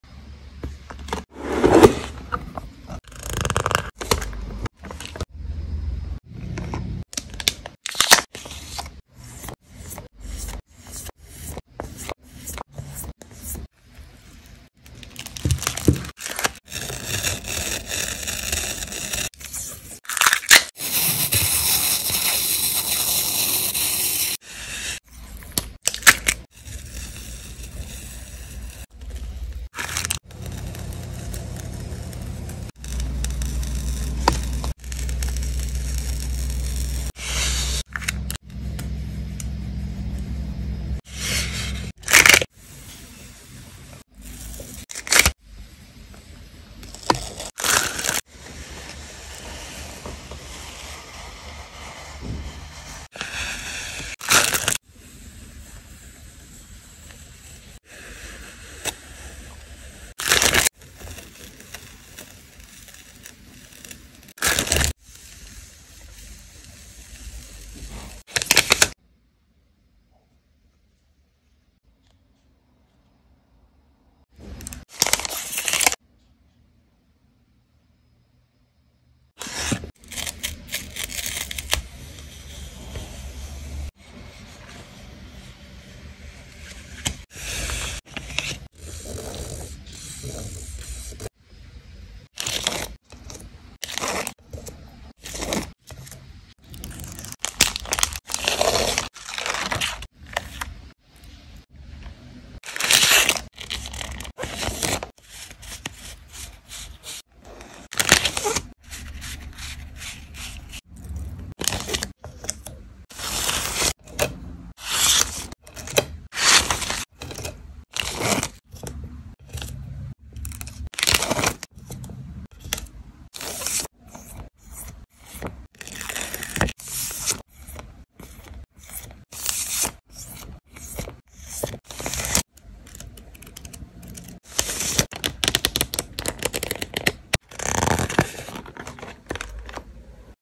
ASMR unboxing journal supplies from @mooboomplanner ✨💖 | Stationery haul 🌷💕
ASMR unboxing beautiful supplies from @mooboomplanner 💖✨

Yeah, there's a lot of PET tapes agaaaiin 😁 But look how gorgeous they are, I just can't put them aside when I order from the Mooboom store, there are so many unusual designs… 🥰 I'm so excited about the pirate tape, pink color looks rather unusually but magical in a pirate theme 😍🦄 Please write your favorite product from this unboxing in the comments 👇🏻☺️